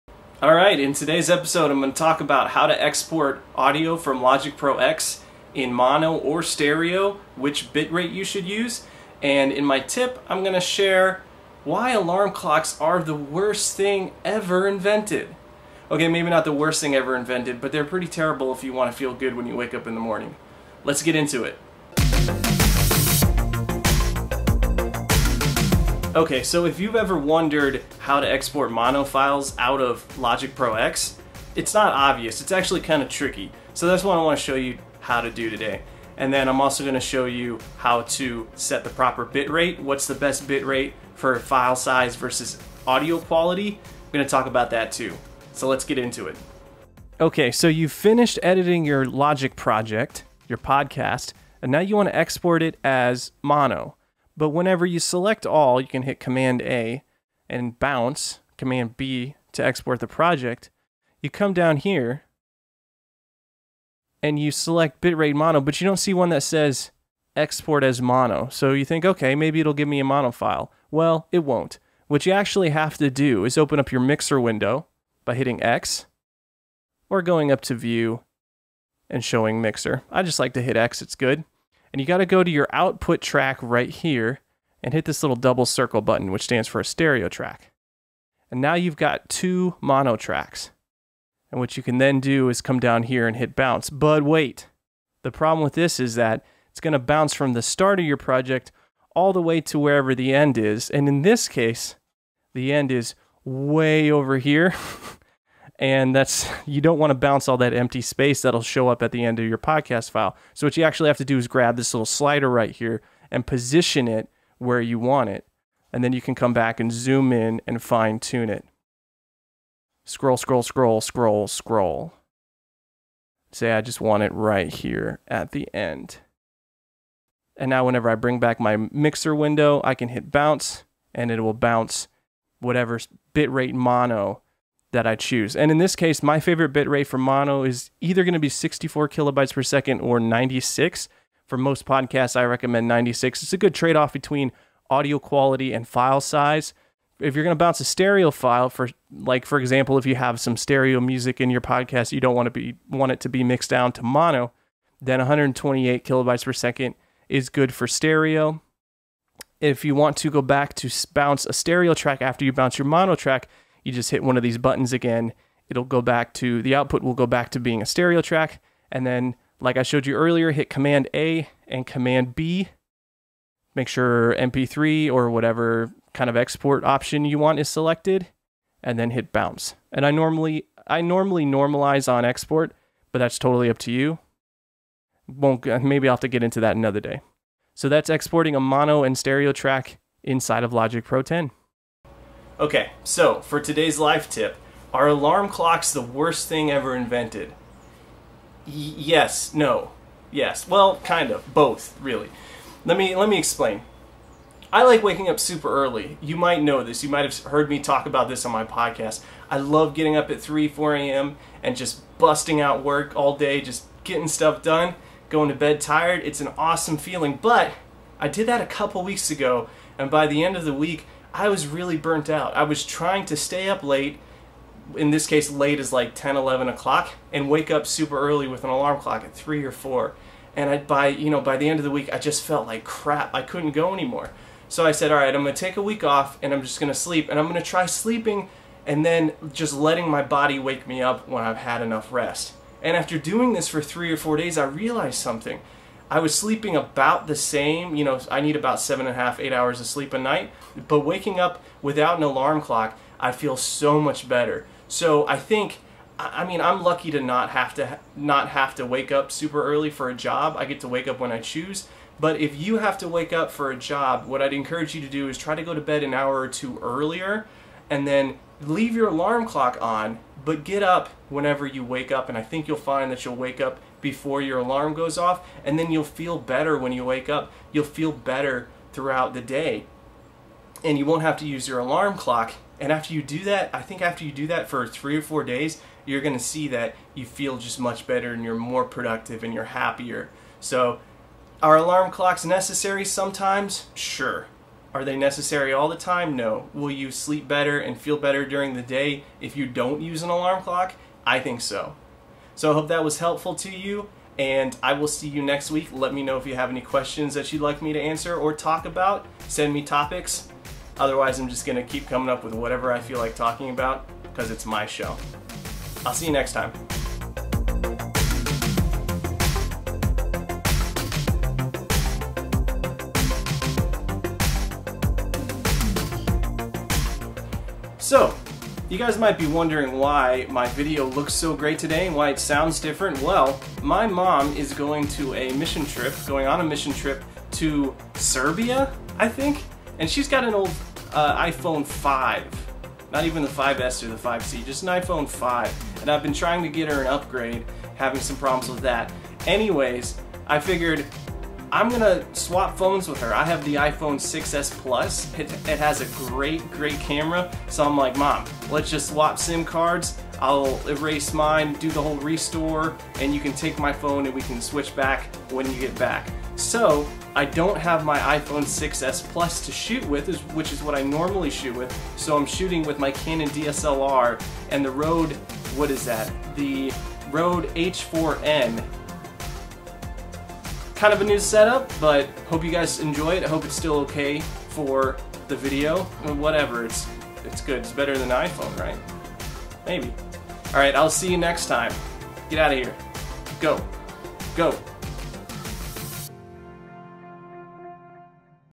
e005: How to Export Mono Files in Logic Pro X
No Dowd About It e005: How to Export Mono Files in Logic Pro X and Why Alarm Clocks are the Worst Thing Ever